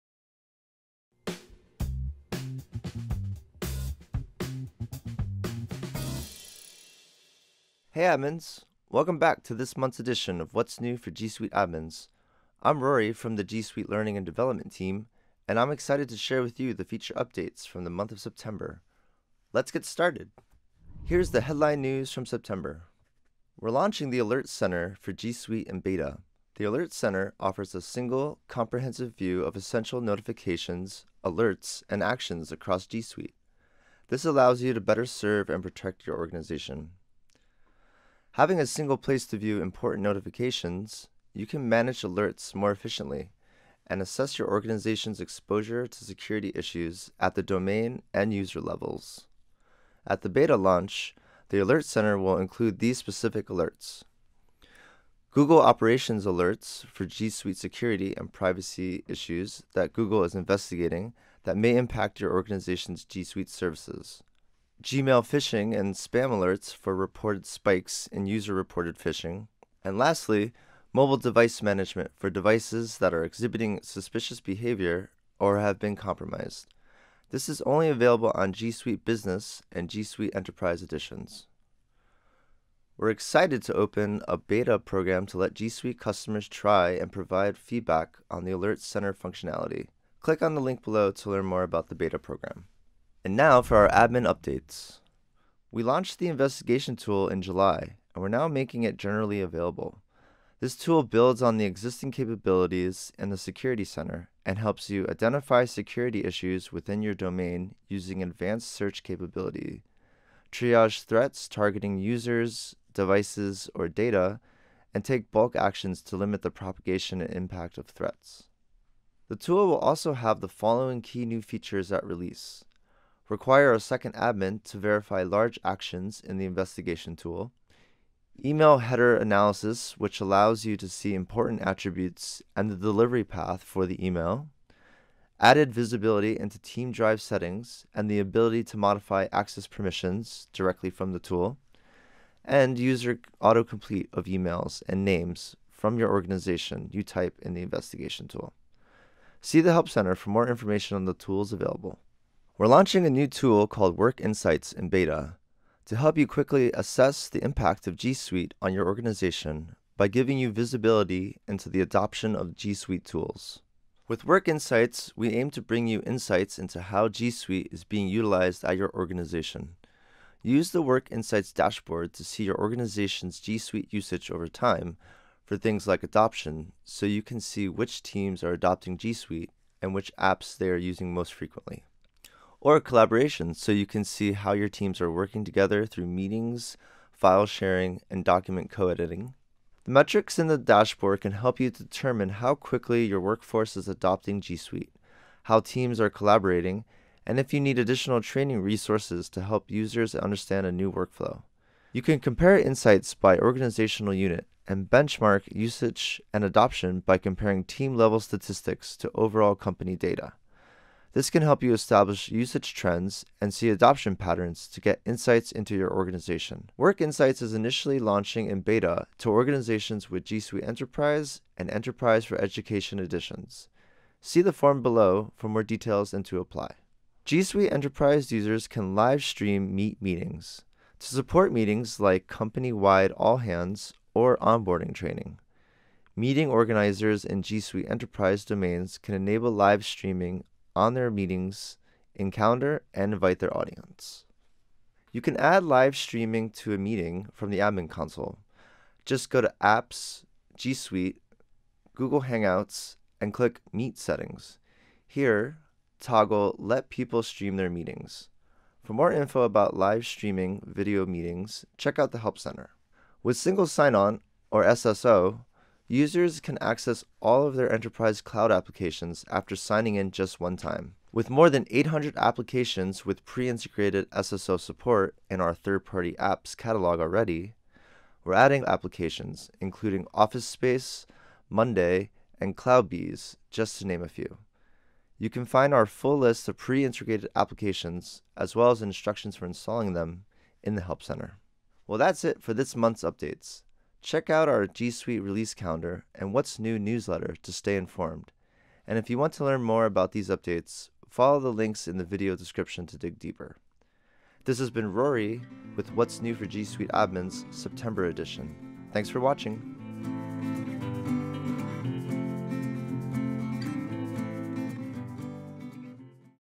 What's New for G Suite Admins? - September 2018 Edition
Get a recap of all the G Suite features relevant to Admins released in September 2018.

Agenda:
- Headline News - The New Alert Center 0:22
- Admin Updates - Investigation tool in the Security Center 1:36
- Admin Updates - Assess the impact of G Suite with Work Insights 2:33
- Admin Updates - Livestreaming meetings 3:47
- Admin Updates - Nine third-party apps added to the G Suite SAML apps catalog 4:23